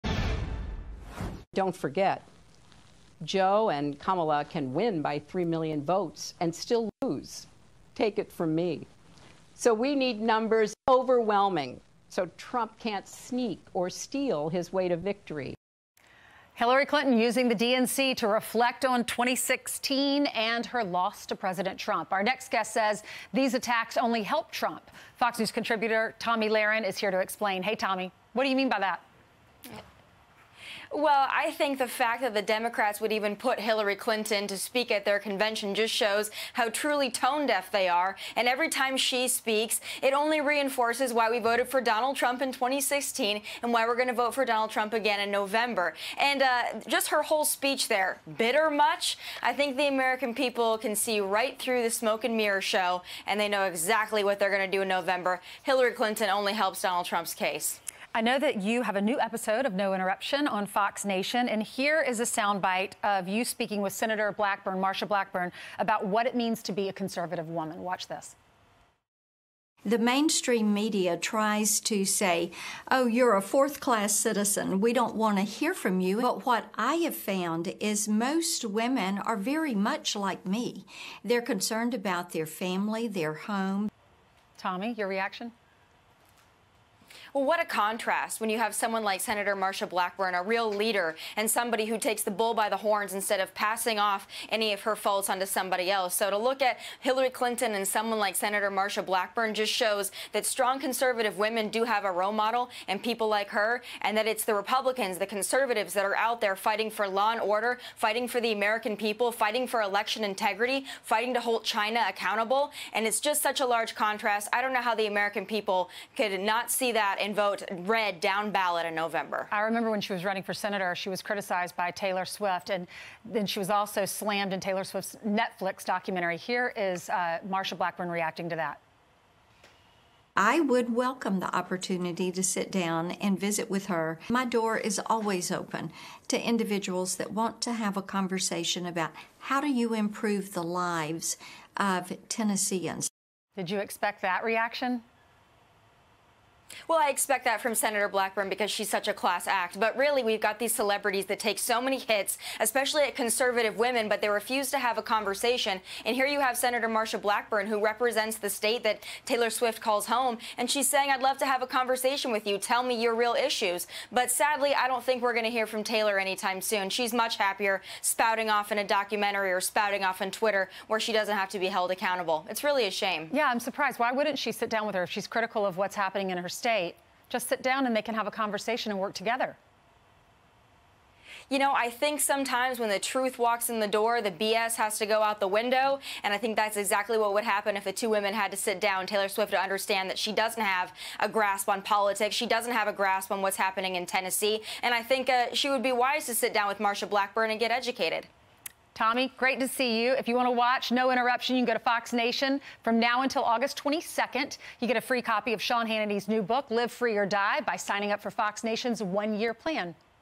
Tomi Lahren calls Hillary Clinton's DNC speech 'tone deaf' and 'bitter'
Fox News contributor Tomi Lahren reacts to night 3 of DNC, previews interview with Sen. Marsha Blackburn on 'No Interruption.'